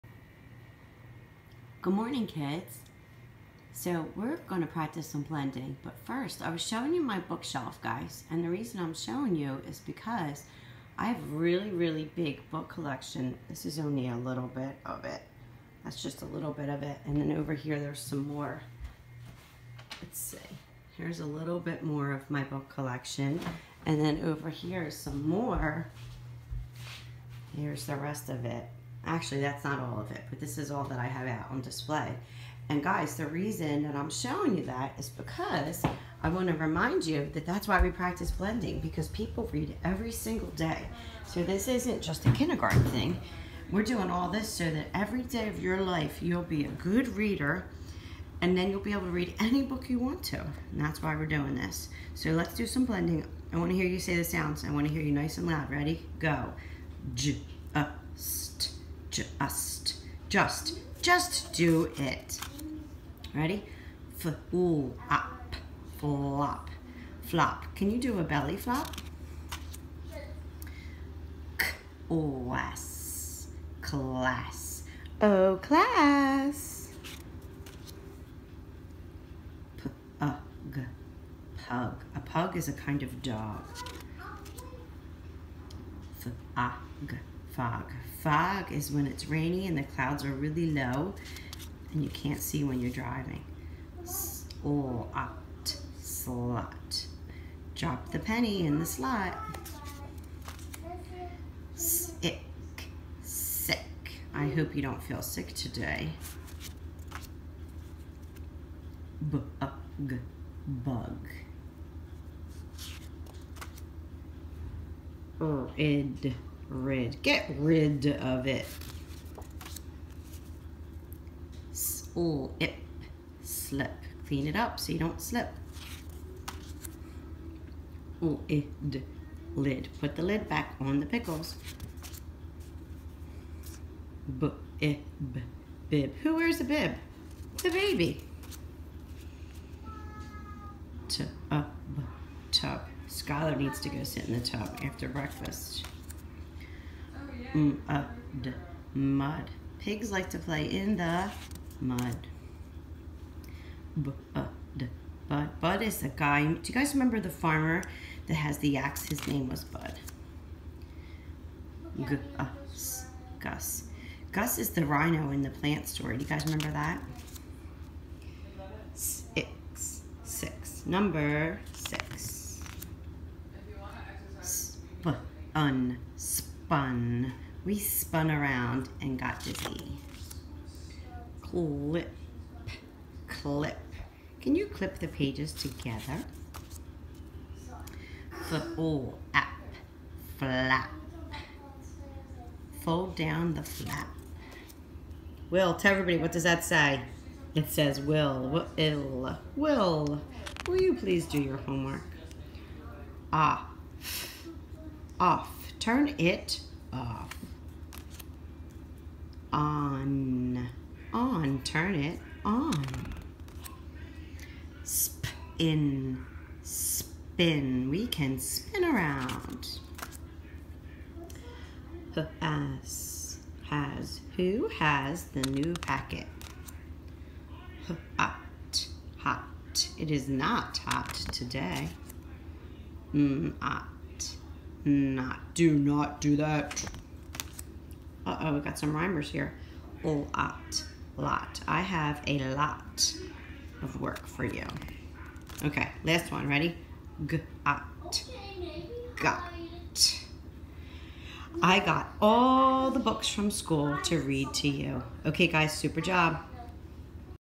Read the words with me! More blending practice!
Blending cvc and ccvc words, all vowels